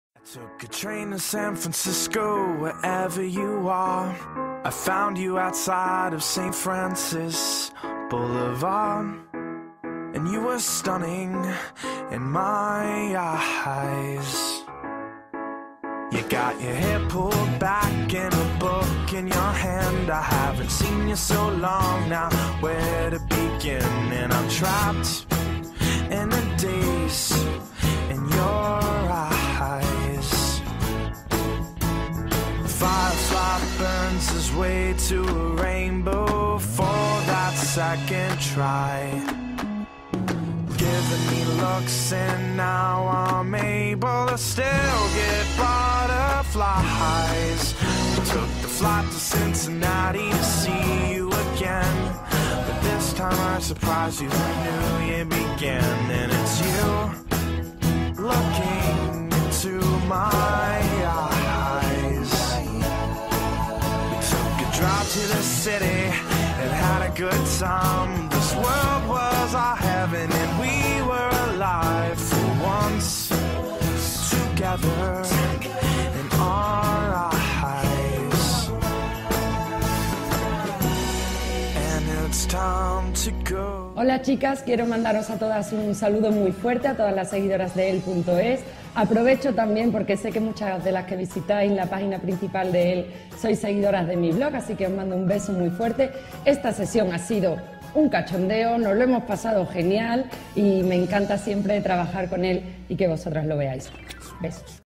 Eva González ~ Elle Spain September 2013
Making of de la sesión de fotos de la presentadora y modelo Eva González para la revista Elle en su edición española de Septiembre de 2013.